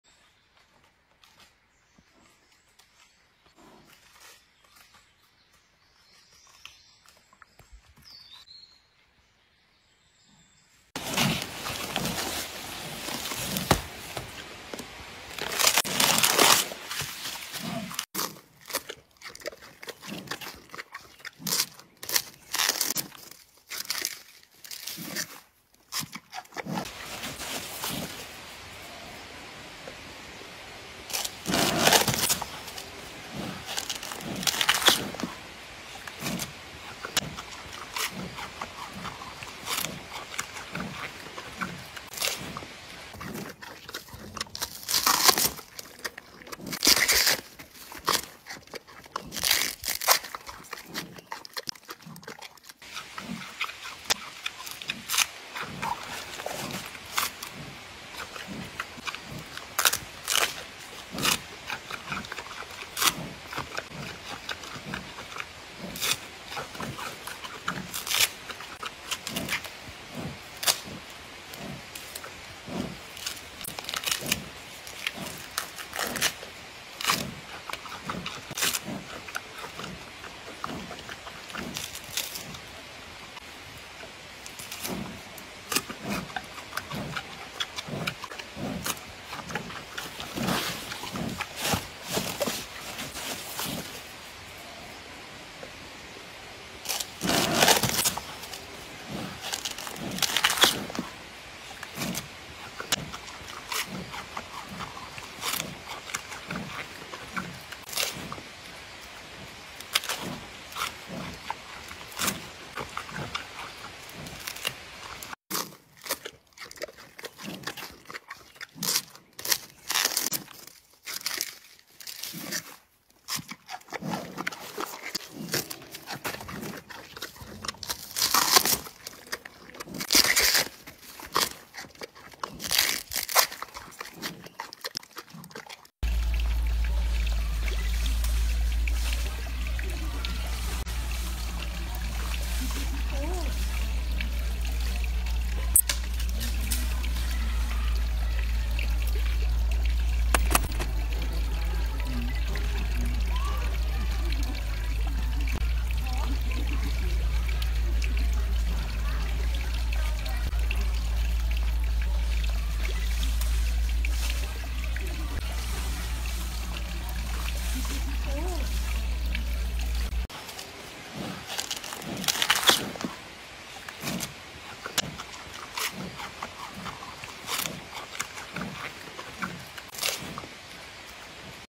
Giant Panda Eating Bamboo ASMR
Video compilation of pandas eating bamboo. The cute big panda is eating a lot of crispy bamboo.